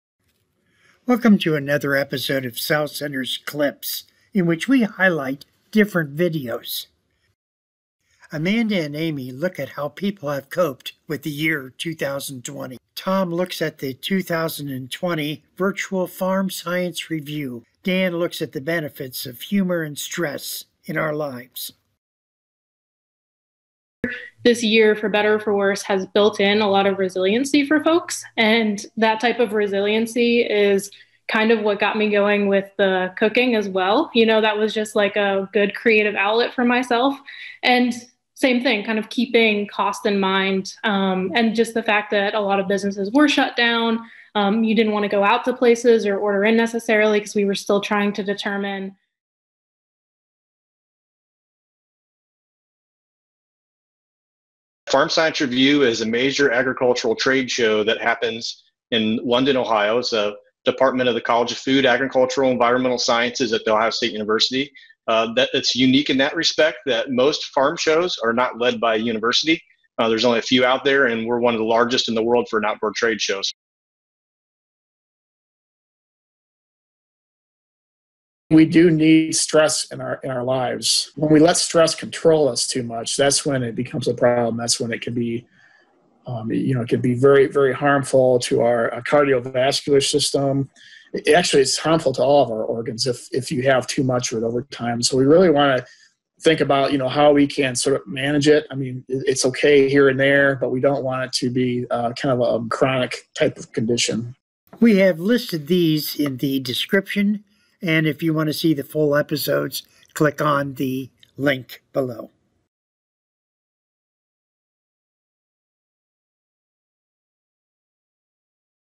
3-SOUTH CENTERS' CLIPS: Coping with 2020, Virtual Farm Science Review and Stress and Humor
3-SOUTH CENTERS' CLIPS:  Coping with 2020,  Virtual Farm Science Review and Stress and Humor

1.     HEALTHY LIVING RESOURCES – Amanda and Amy

2. SOUTH CENTERS CHAT with Tom Worley

3. FOOD, NUTRITION and WELLNESS with Dan Remley